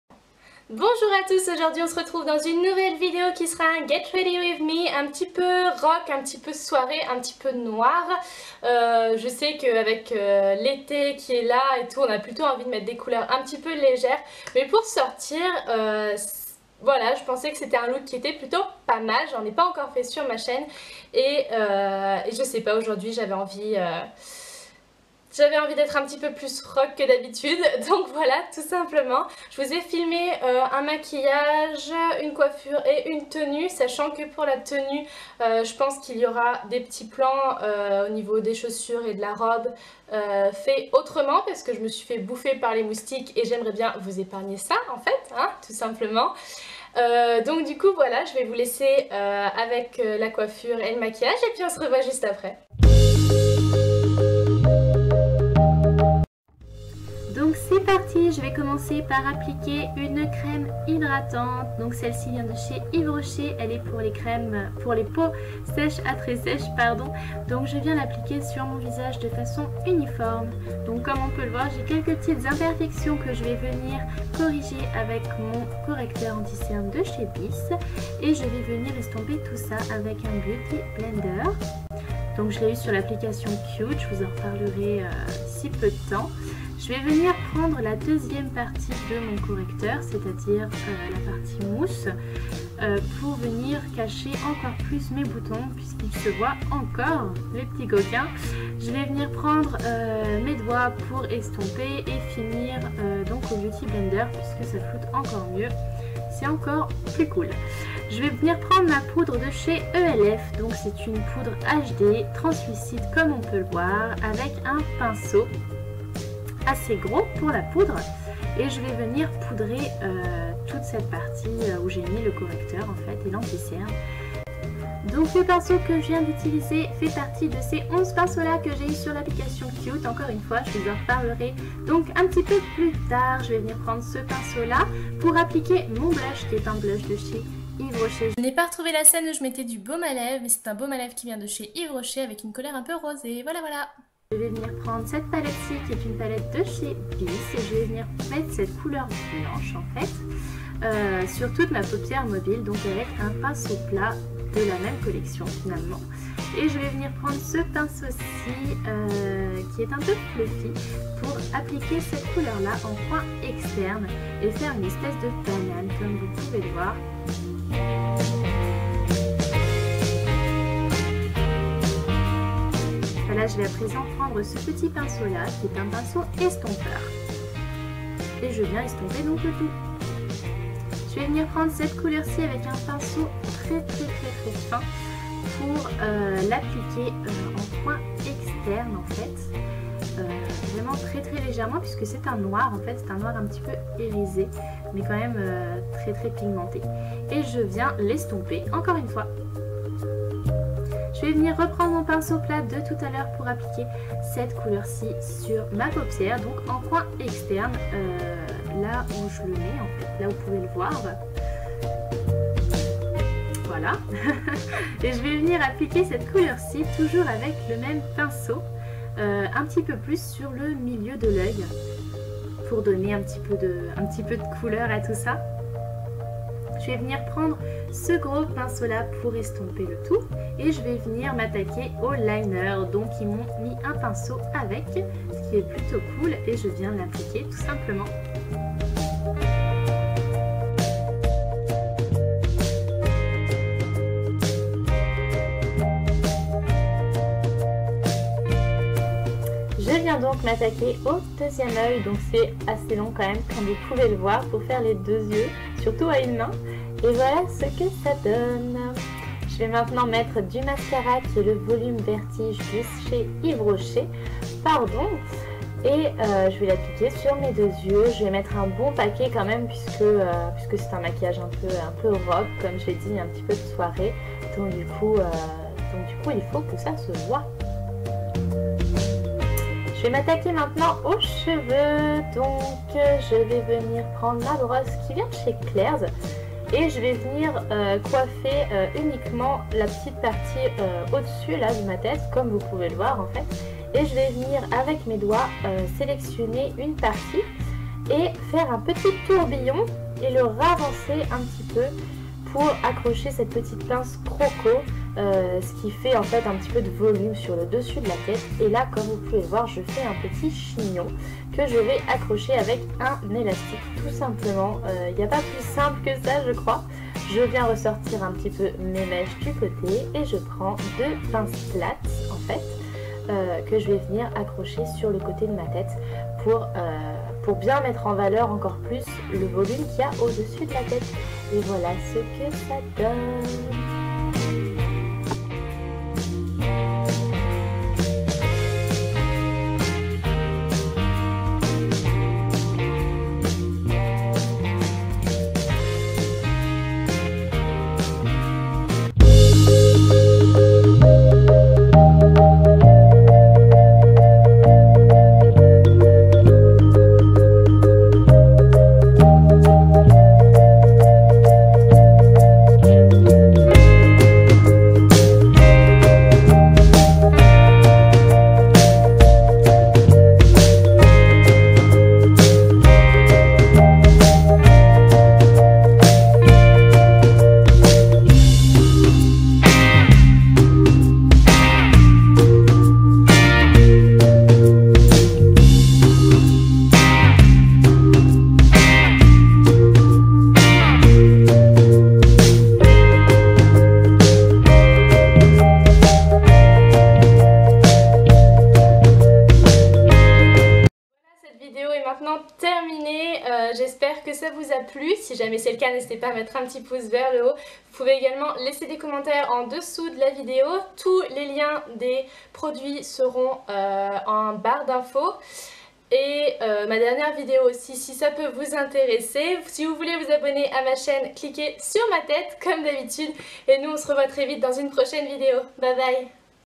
GRWM #1 ; Look Rock pour l'été
Bonjour à tous, je vous retrouve aujourd'hui pour un Get Ready With Me spécial Rock en été, qui convient pour les soirées.

Produits :
Crème Hydratante Yves Rocher
Anti cerne BYS
Poudre HD ELF
Pinceaux Cute
Baume à Lèvres Yves Rocher
Palette BYS
Liner ELF
Mascara Yves Rocher
Blush Yves Rocher

Tenue :
Robe MIM
Sac Chic Génération
Chaussures Gémo

Bye !